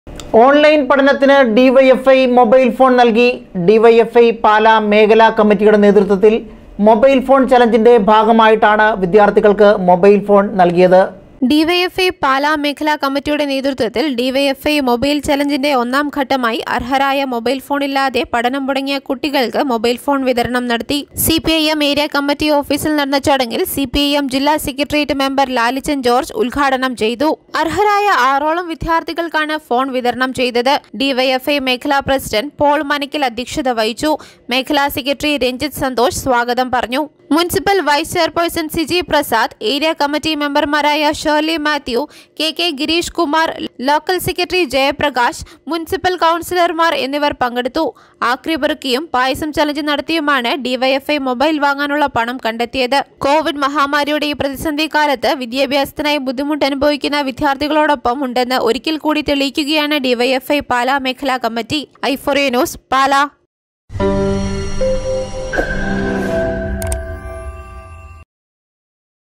ഓണ്‍ലൈന്‍ പഠനത്തിന് മൊബൈല്‍ ഫോണ്‍ നല്‍കി DYFI
ഓണ്‍ലൈന്‍ പഠനത്തിന് DYFI മൊബൈല്‍ ഫോണ്‍ നല്‍കി. ഡിവൈഎഫ്‌ഐ പാലാ മേഖല കമ്മിറ്റി യുടെ നേതൃത്തില്‍ മൊബൈല്‍ ചലഞ്ചിന്റെ ഭാഗമായിട്ടാണ് വിദ്യാര്‍ഥികള്‍ക്ക് മൊബൈല്‍ഫോണ്‍ നല്‍കിയത്.